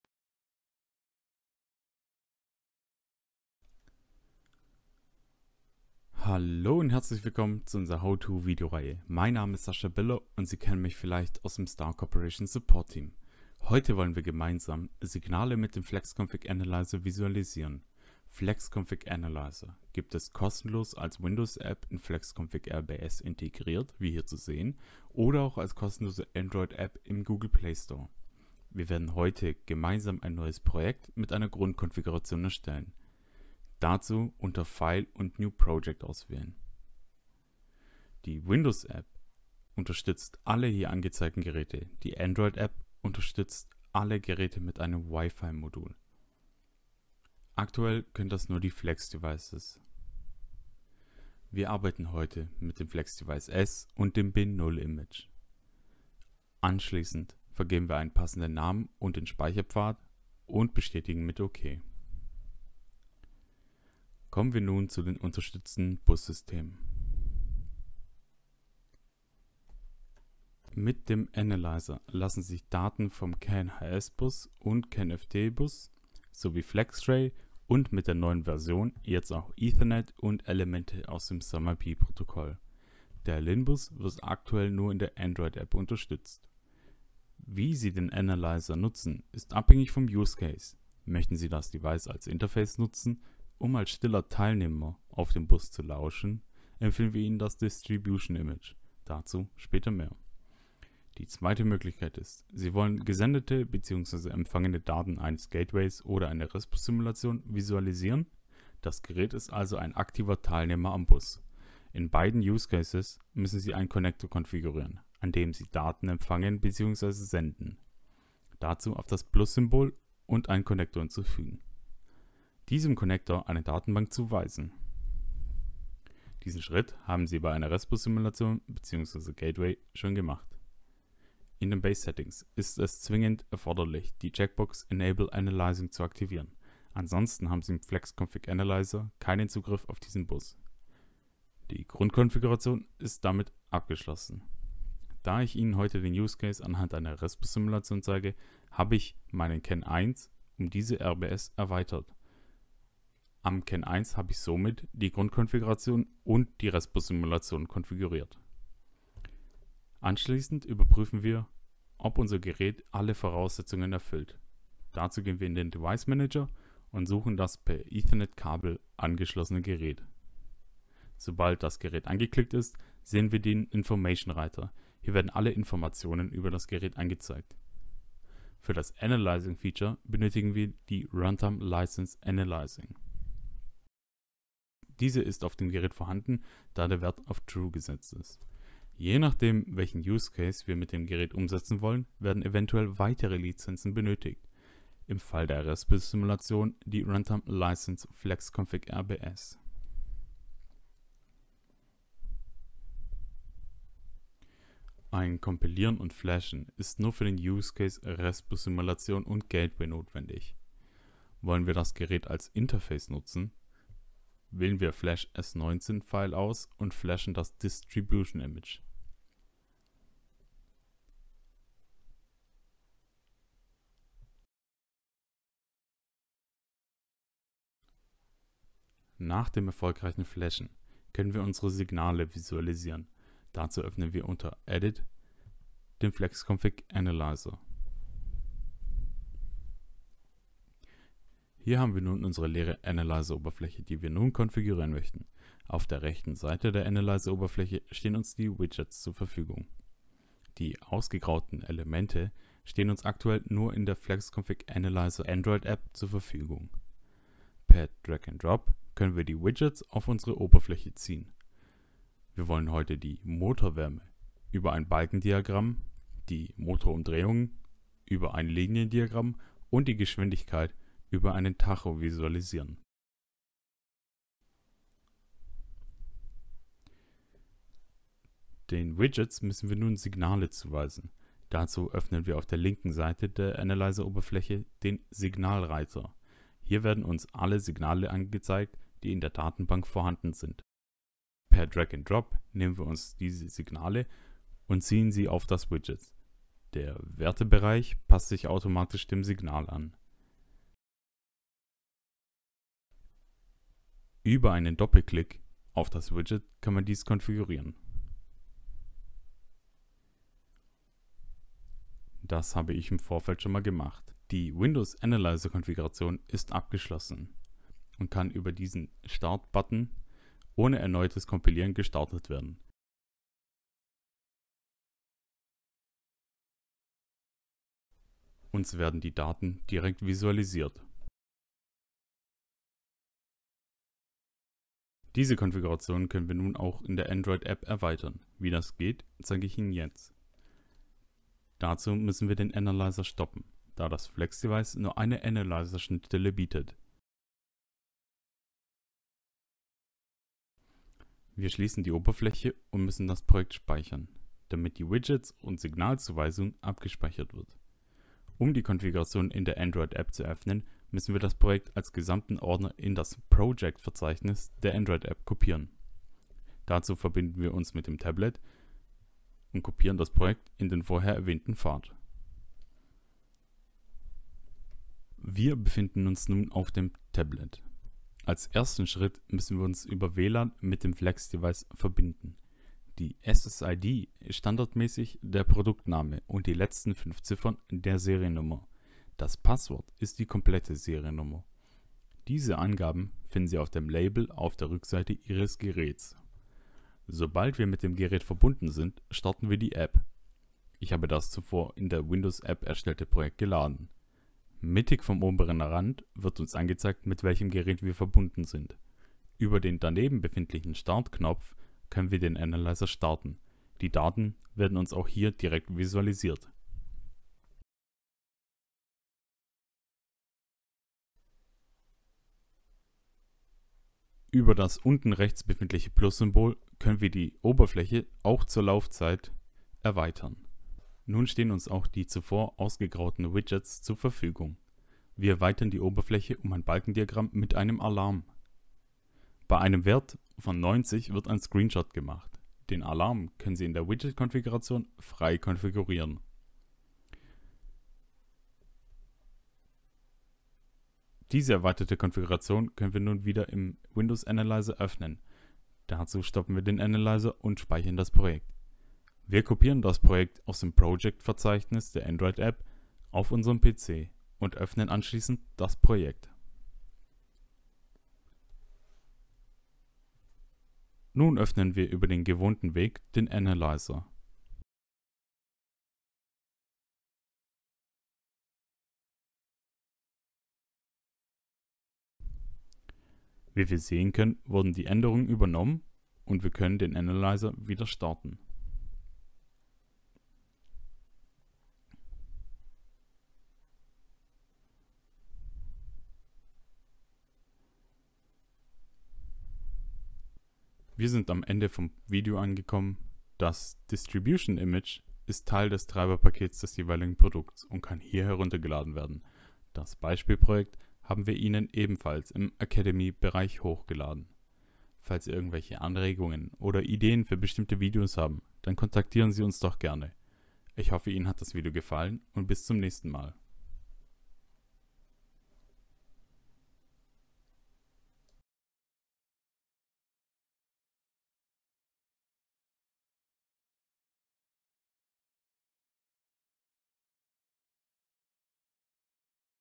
How-to: Datenvisualisierung mit dem FlexConfig Analyzer
In diesem Video zeigen wir Ihnen, wie Sie Konfigurationen zur Bus-/Netzwerk-Analyse mit dem FlexConfig Analyzer erstellen können.
Über eine Windows und Android Oberfläche lassen sich mit dem FlexConfig Analyzer grafische Oberflächen erstellen, über welche Signale und Datentypen visualisiert werden können.